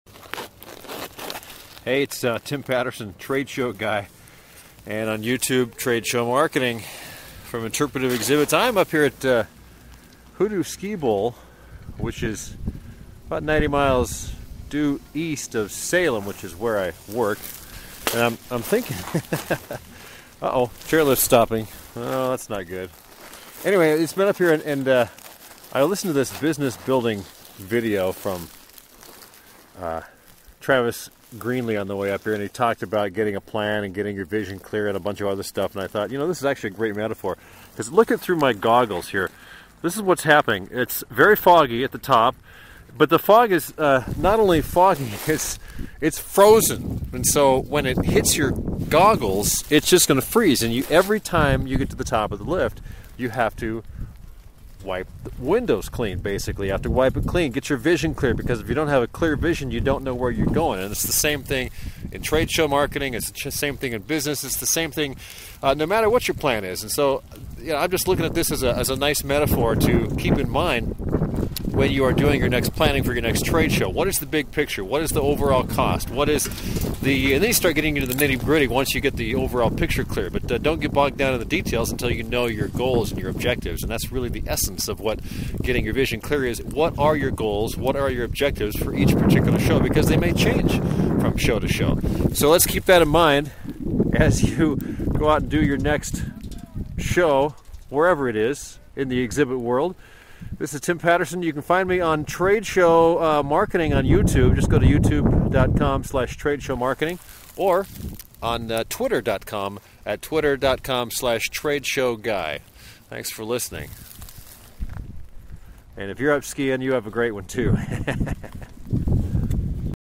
Tradeshow Marketing Tips
Tim Patterson of Interpretive Exhibits hits the ski slope and takes a moment in the freezing cold to share a tradeshow marketing tip.

He even makes reference to Travis Greenlee!